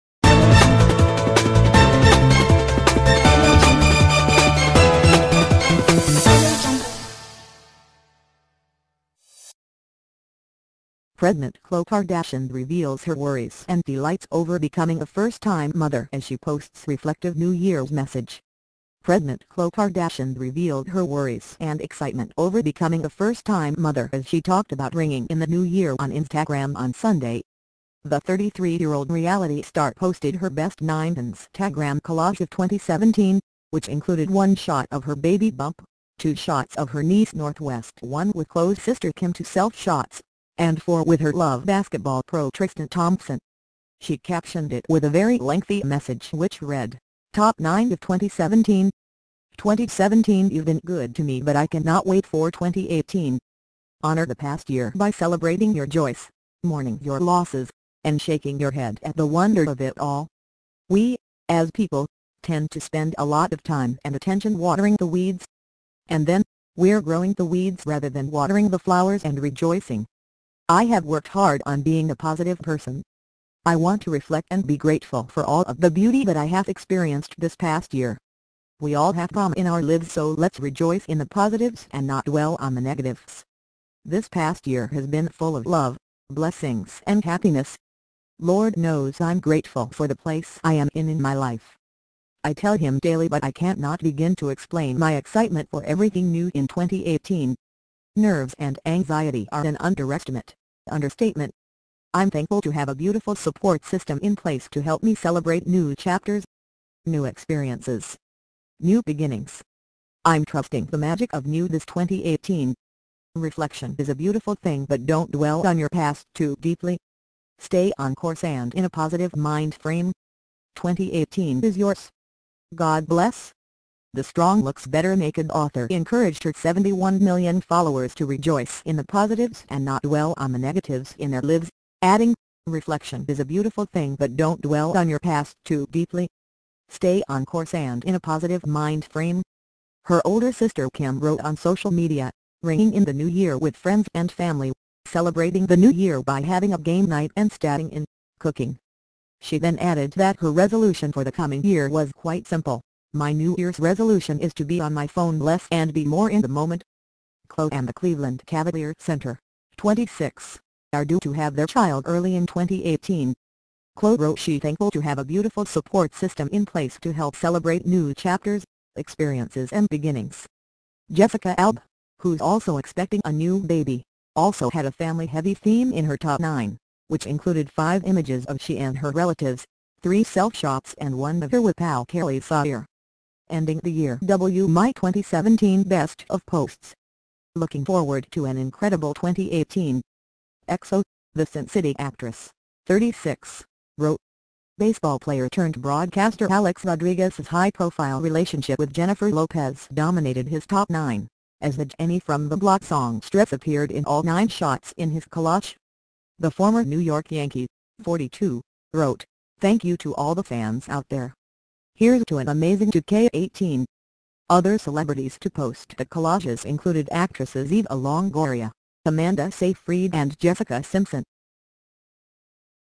Pregnant Khloe Kardashian reveals her worries and delights over becoming a first-time mother
Pregnant Khloe Kardashian revealed her worries and excitement over becoming a first-time mother as she talked about ringing in the New Year on Instagram on Sunday.